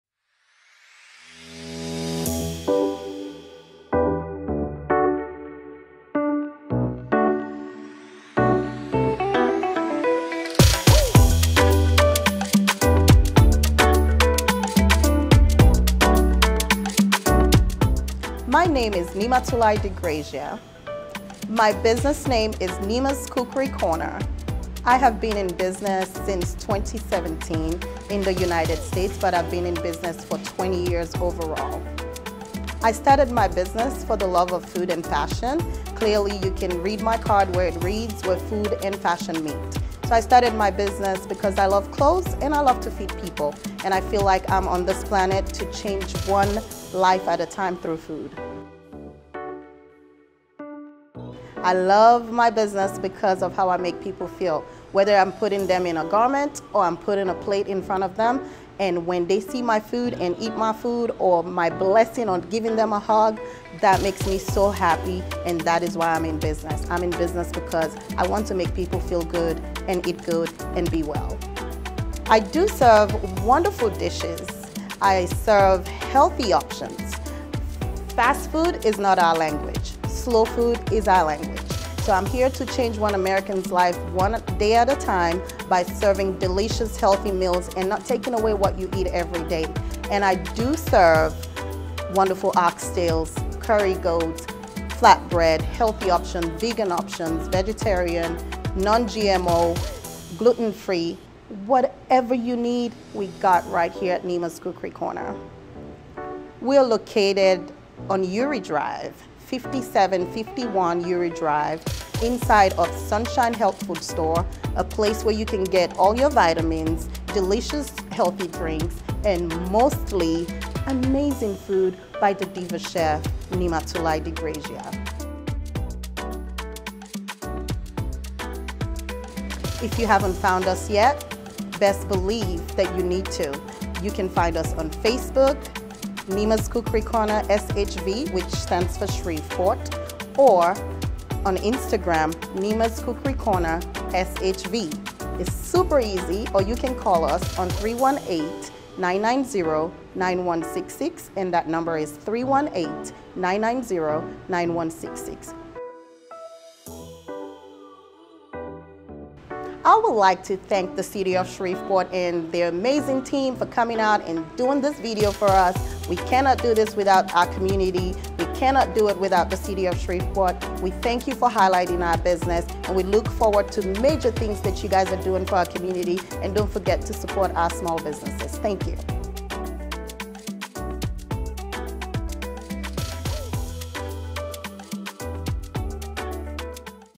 Shreveport's Business Spotlight Niema's Cookery Corner
This month Business Spotlight features Niema’s Cookery Corner Shv! Nestled within Sunshine Health Market,at 5751 Youree Drive, this awesome business is a hidden gem renowned for its delectable and authentic cuisine. Their menu boasts of mouthwatering options like fufu served with Egusi, okra, or peanut, along with tempting choices such as flatbread portobello mushroom or jerk chicken. Another highlight is their Caribbean Turkey wing brown stew.

For an unforgettable culinary adventure with Chef’s Matthew and Niema DiGrazia and their amazing team, make your way to Niema's Cookery Corner Shv.